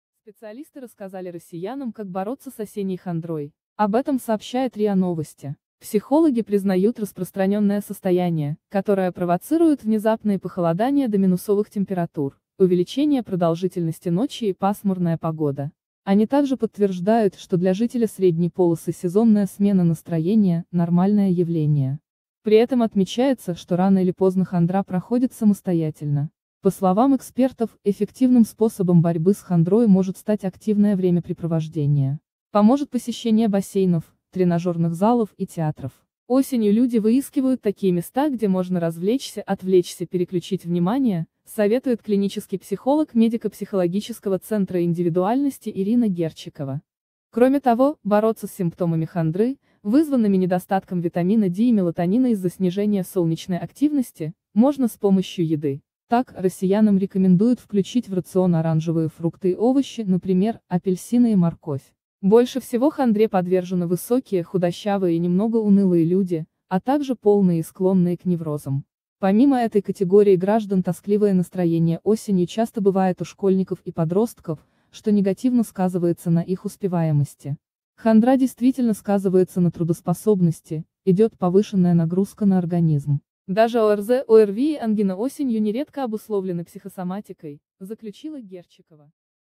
Раскрыты эффективные способы борьбы с осенней хандрой россиян
Специалисты рассказали россиянам, как бороться с осенней хандрой.